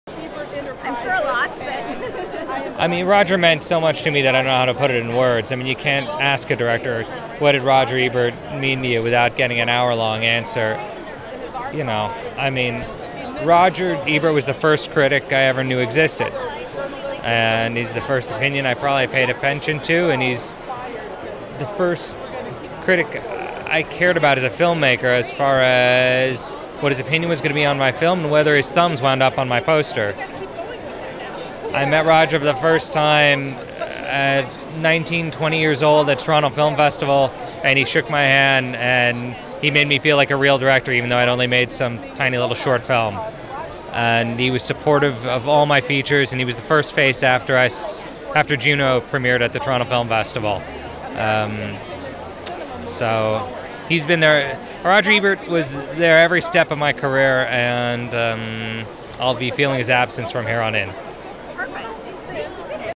INTERVIEW - Jason Reitman on Roger Ebert at 3rd Annual 'C...
INTERVIEW - Jason Reitman on Roger Ebert at 3rd Annual 'Celebrate Sundance Institute' Los Angeles Benefit Honoring Roger Ebert and Ryan Coogler on 6/6/13 in Los Angeles, CA .